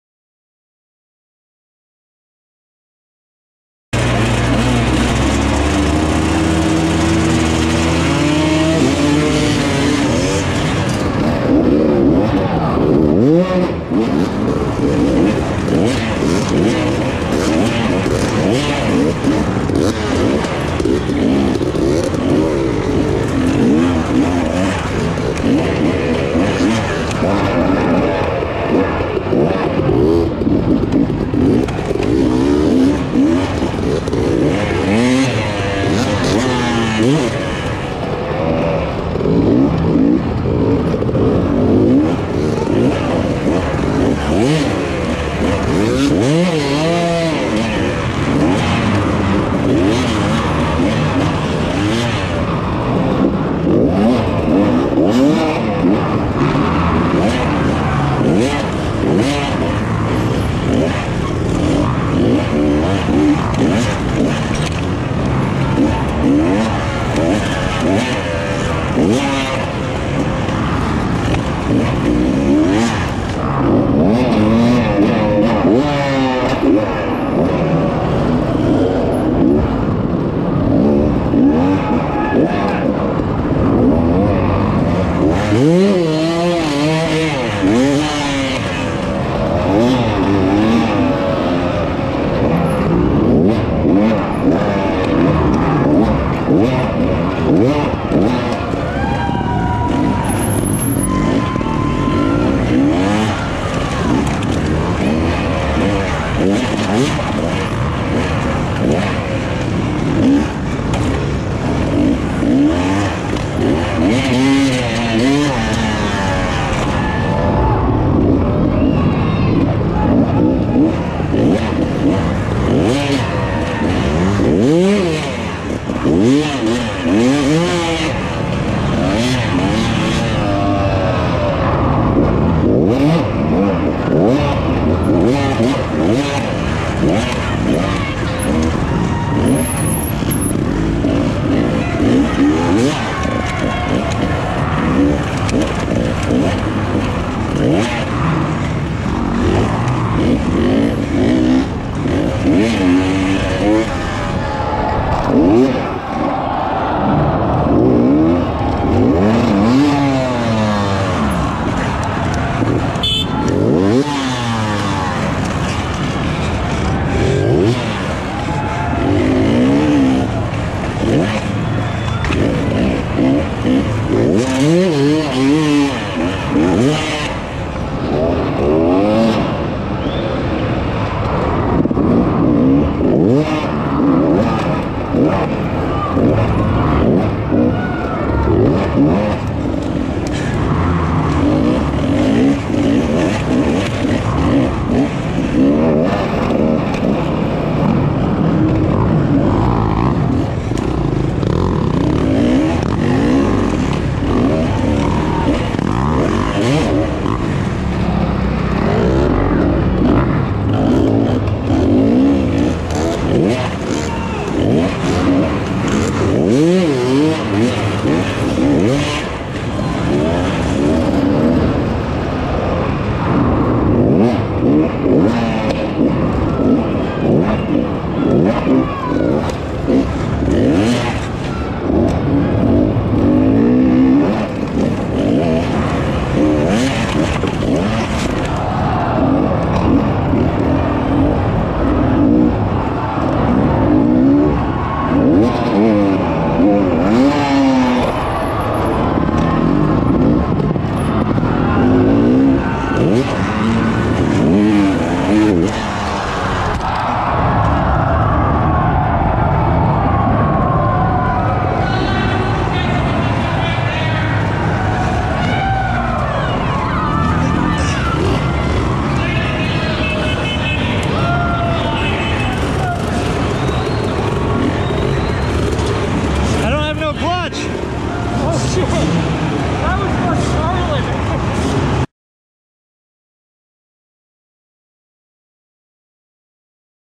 Extreme Enduro 2022 Waterloo Iowa
The Extreme Enduro 2022 in Waterloo Iowa put on by Seven Four Promotions. I finished first in the first heat race and then had a little slip up and had some clutch issues the last two laps of the main and ended up getting second losing by just a front tires distance and whiskey throttling off the finish line jump at the end when i was trying to get the pass.